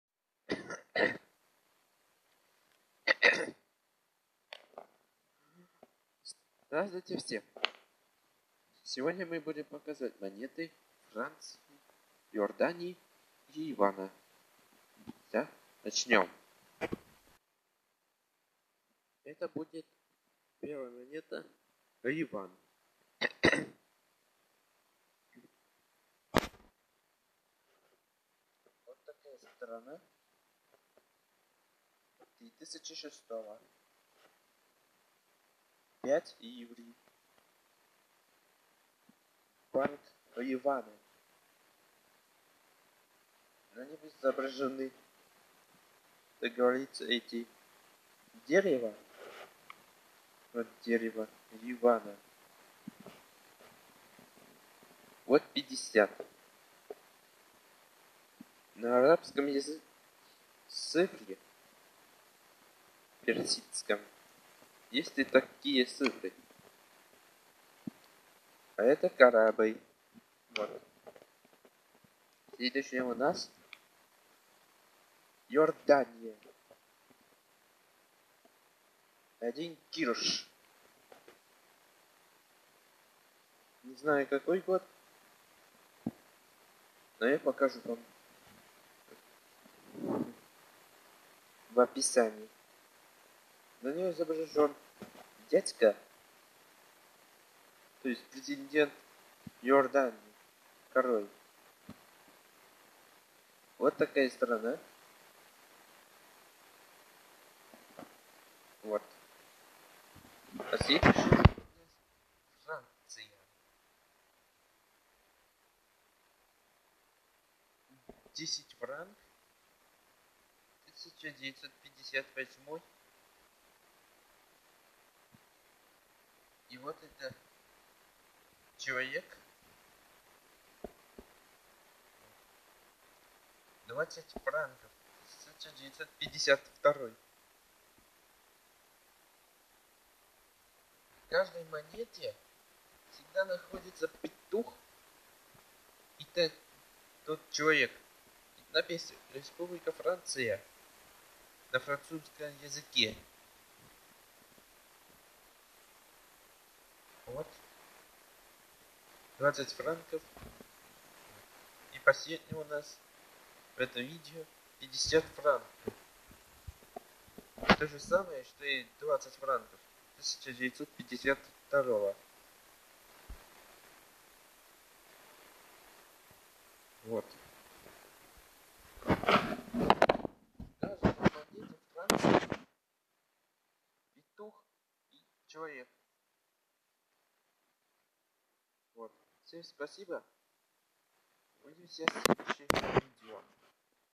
Монеты Ливана, Иордании и Франции
Монеты:
Ливан: 50 ливров 2006
Иордания: 1 кирш 2000
Франция: 10 франков 1958, 20 франков и 50 франков 1952.